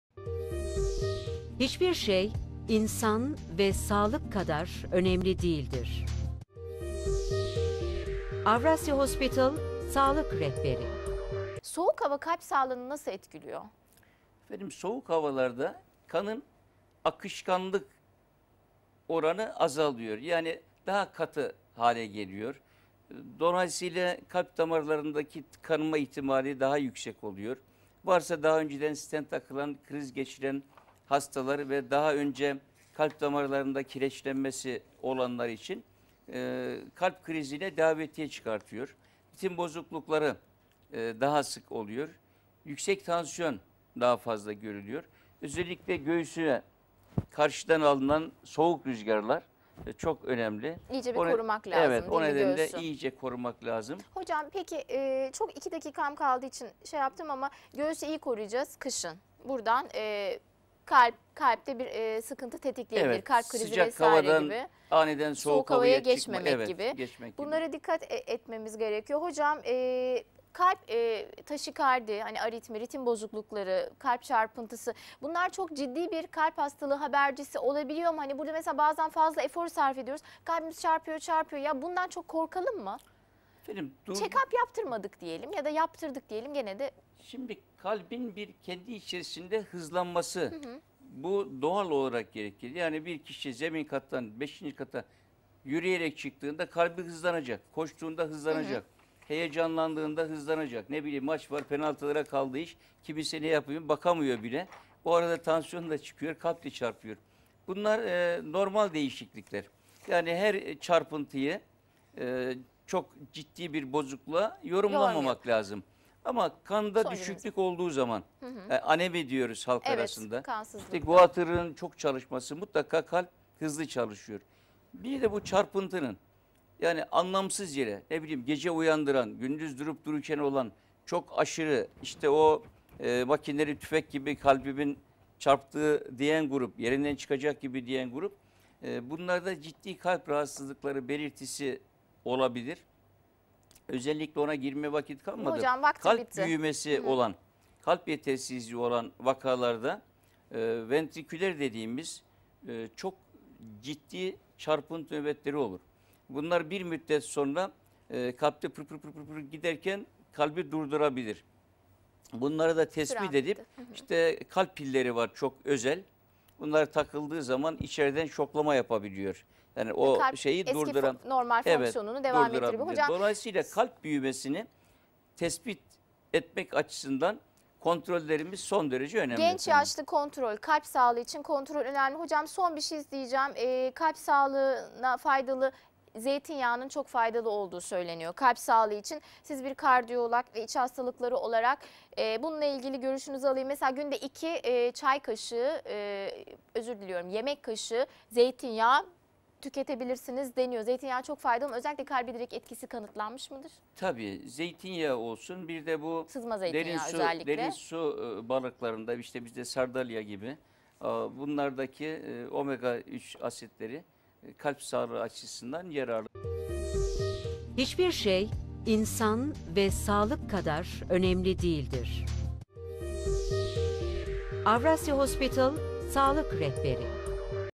Soğuk Hava Kalp Hastalıklarını Nasıl Etkiler ?
Soğuk Hava Kalp Hastalıklarını Nasıl Etkiler ? Avrasya Hospital Kardiyoloji ve İç Hastalıkları Uzmanı Prof. Dr. Ali Bayram
Bilgilendiriyor. Avrasya Hospital Sağlık Raporu Programı